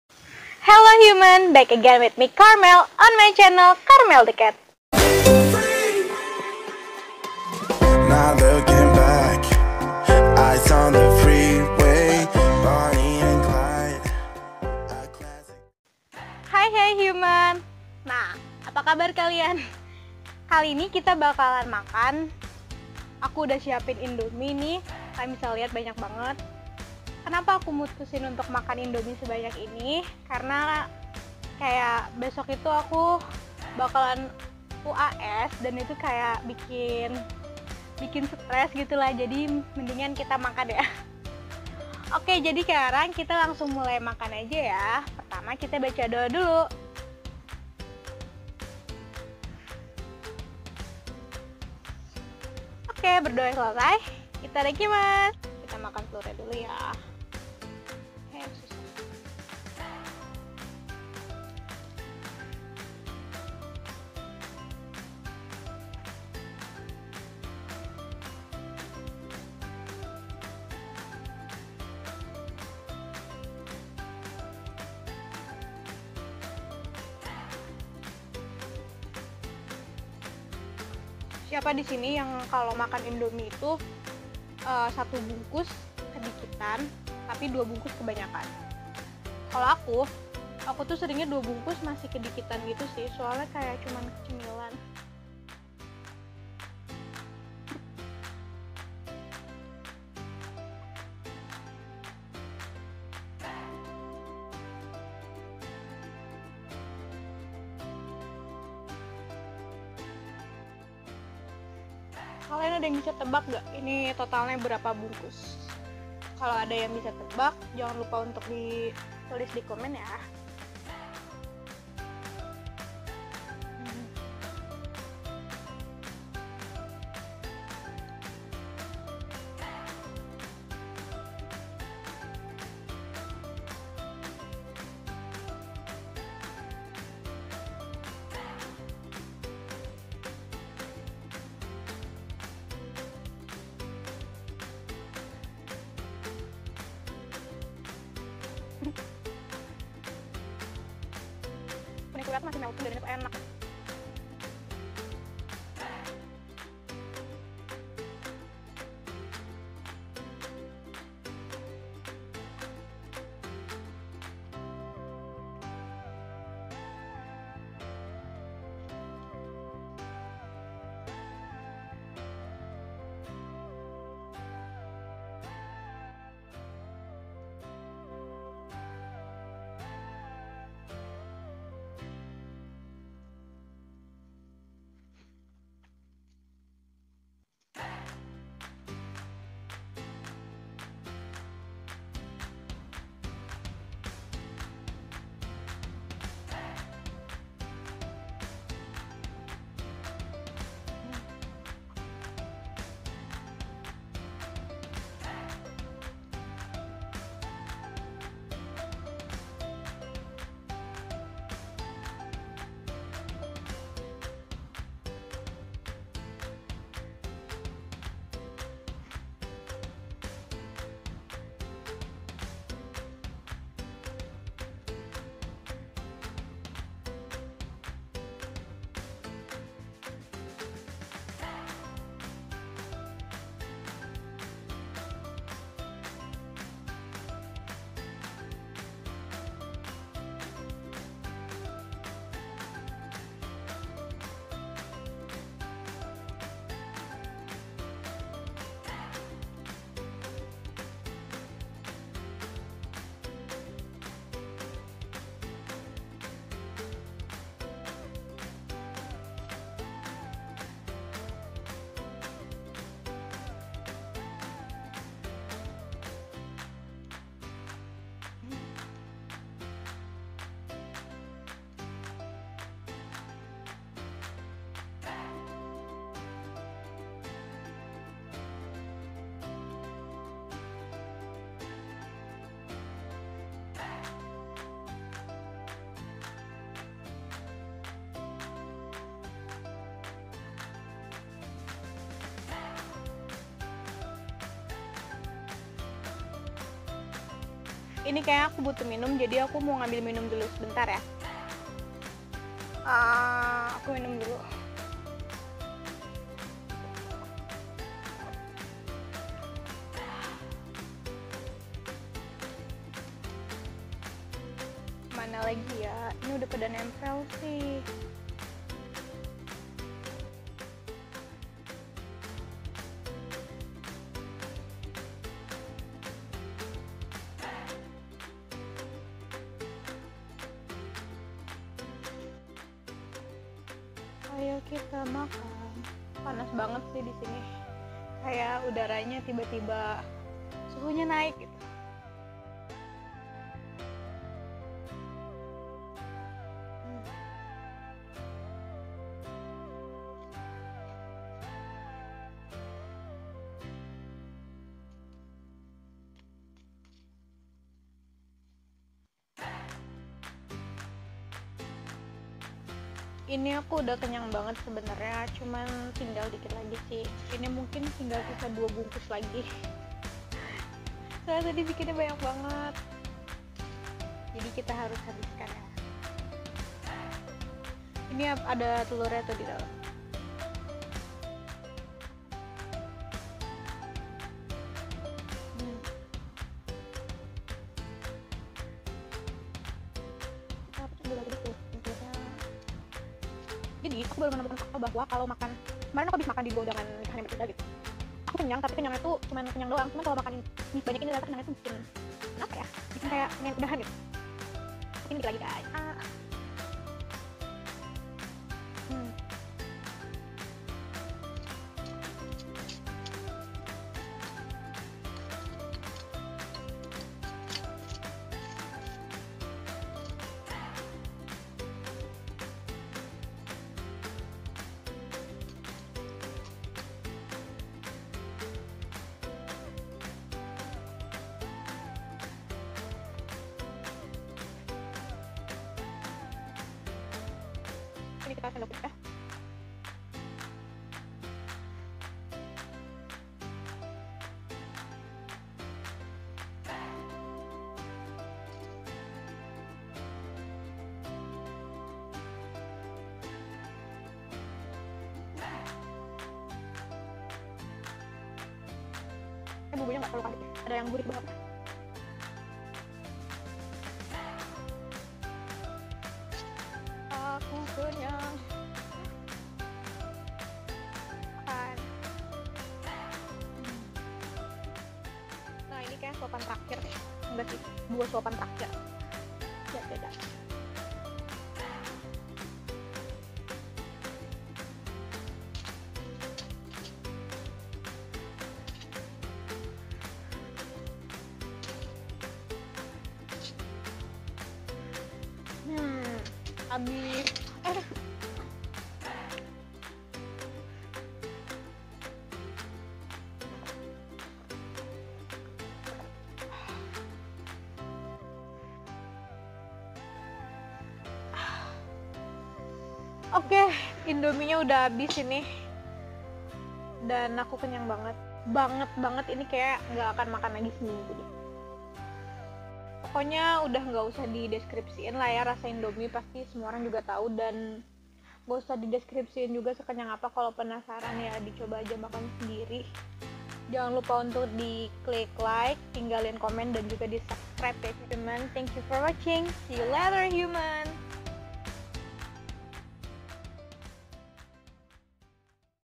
MAKAN PORSI JUMBO! Mukbang Indomie Goreng porsi se-RT + 4 Telur
Siapa pernah makan mie indomie porsi jumbo kaya gini?
Mukbang mie indomie porsi besar gini bikin kenyang parah loh, hooman.

See you later, Hooman!